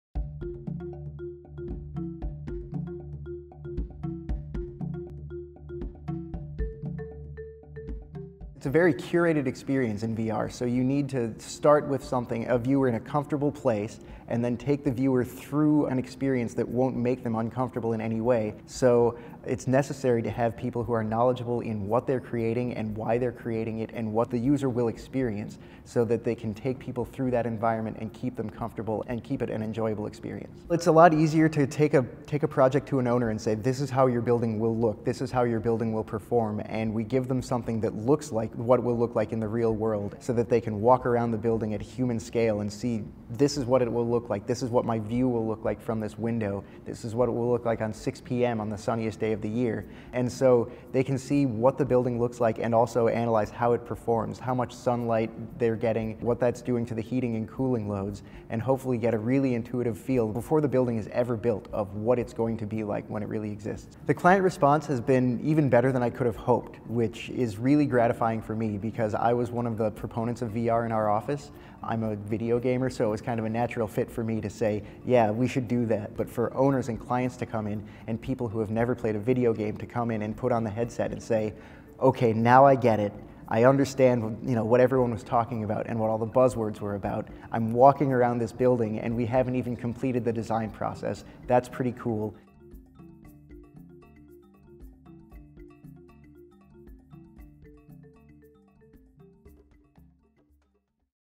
VR Experiences for Understanding Design
In a presentation at AU Las Vegas, Andrew Leavitt discusses using Revit energy modeling and Green Building Studio cloud-based service to improve sustainability and produce better buildings. This includes a demonstration of a custom Dynamo mass-making tool that translates a design intent spreadsheet into Revit masses, along with a beginners introduction to conceptual energy modeling. Leavitt uses real-world examples to demonstrate an energy modeling process, and includes comparisons with other common energy modeling software to demonstrate the accuracy and effectiveness of Revit energy modeling.